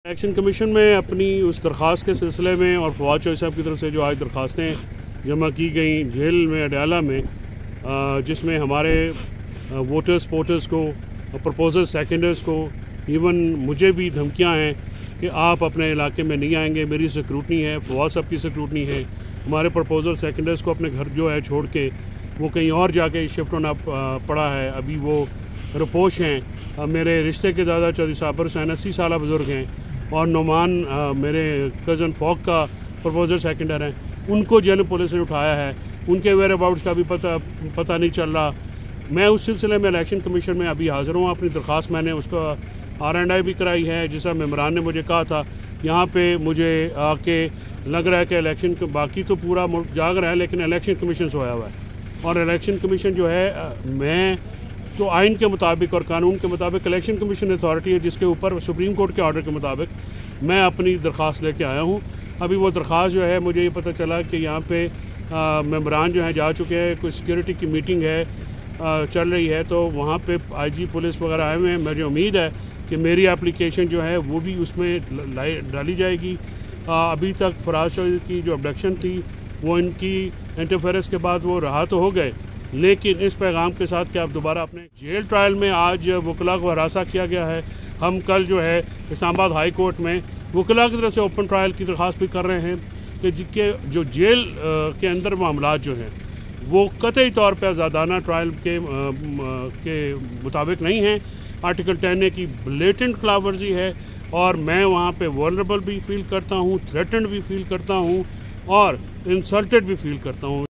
Faisal Chaudhary lawyer of Fawad Chaudhary file application in ECP | Pak Upadtes | December 27,2023.
Myself, Fawad Chaudhary, family are threatened for run in election, Faisal Chaudhary.